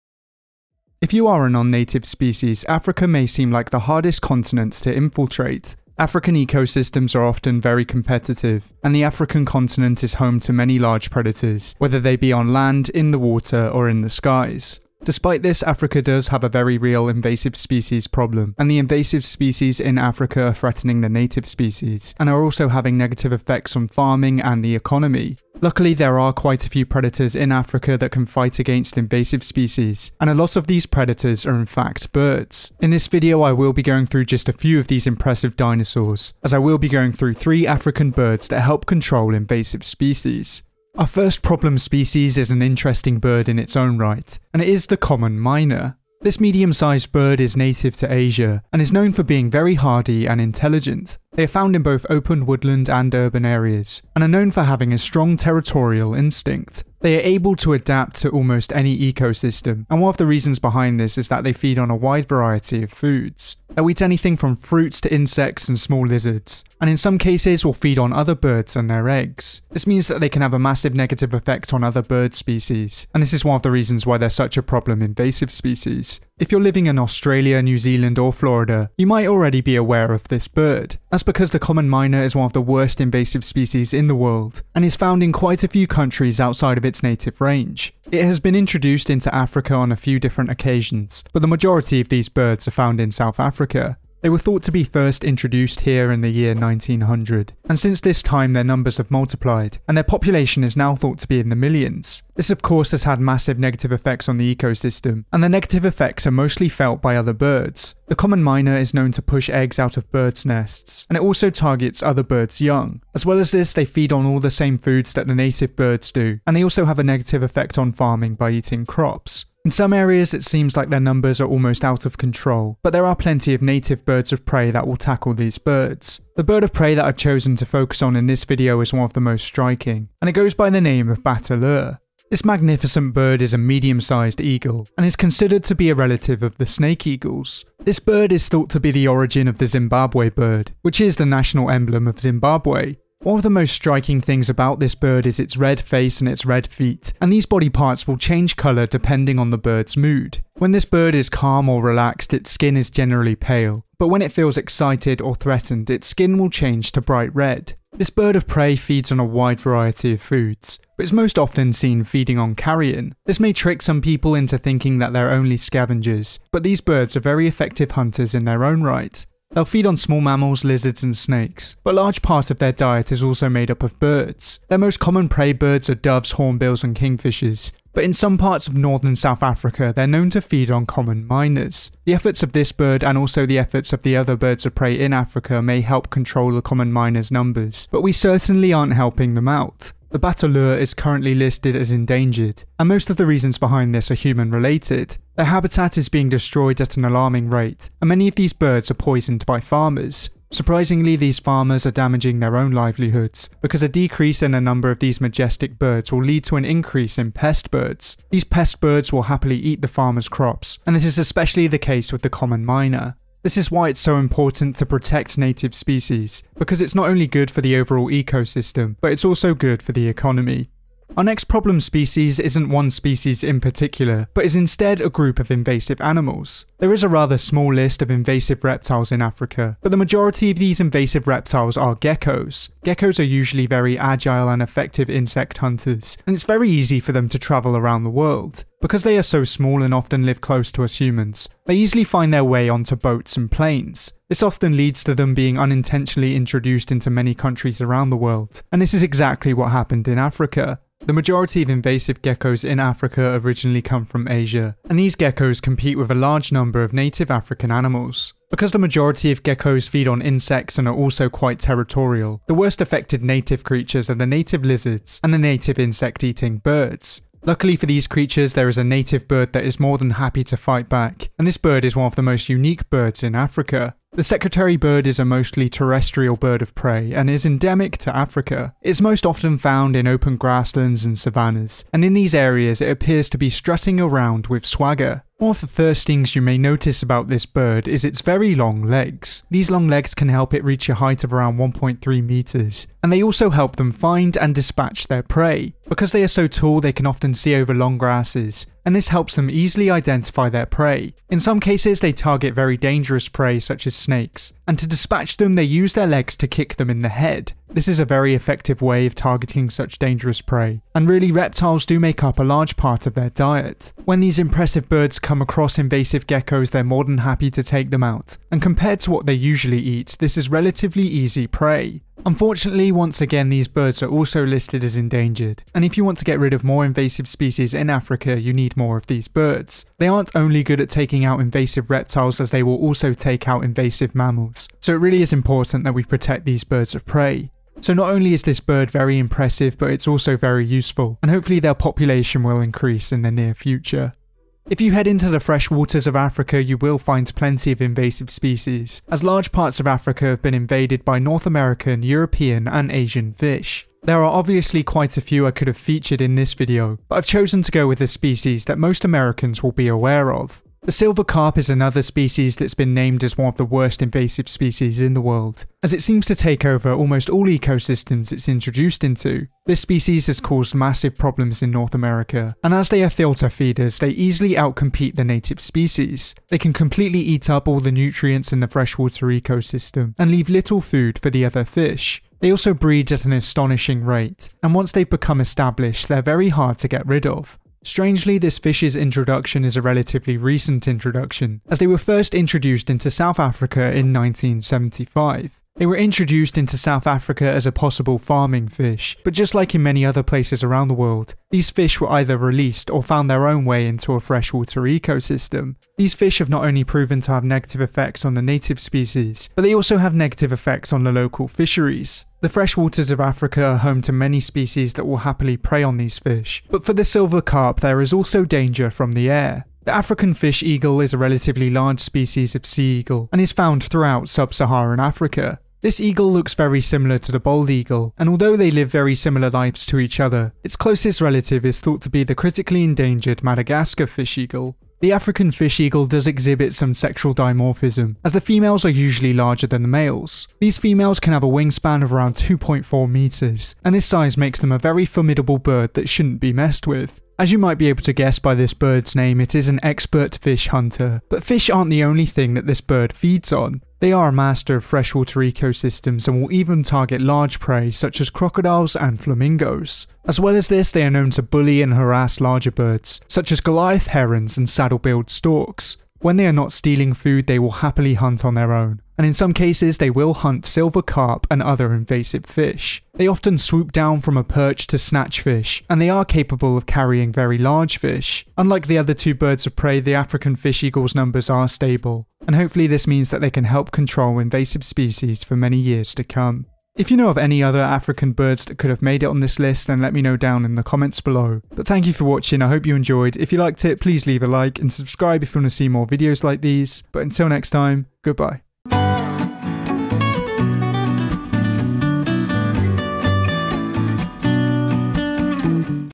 3 African Birds That Hunt And Fight Back Against Invasive Species
If you are a non native animals Africa may seem like a very hard continent to infiltrate. Strangely there are plenty of invasive species on the African continent despite the fact that many of Africa's ecosystems are very competitive. Luckily there are plenty of native animals that will fight back against invasive species and a lot of these animals are birds. In this video i will be going through just a few of these dinosaurs as i will be going through 3 African birds that fight back against invasive species.

Chapters
0:00 Introduction
0:41 Bateleur vs Common Myna
3:41 Secretarybird vs Geckos
5:44 African Fish Eagle vs Invasive Carp

Attributions

Bateleur images:
Tambako The Jaguar

African fish eagle images:
Peter Steward

Secretarybird images:
Wildlife Wanderer
Ninara
Mathias Appel
(CC0 1.0)

Silver carp images:
NOAA Great Lakes Environmental Research Laboratory
Invasive Carp Regional Coordinating Committee
Tino Strauss
LouisvilleUSACE
U.S. Army Corps of Engineers

Common myna images:
T G Santosh

Bateleur footage:
Bernard DUPONT

Secretarybird footage:
Bernard DUPONT

Gecko images:
jaime alejandro montañez mendez

Madagascar fish eagle image:
Brendan Ryan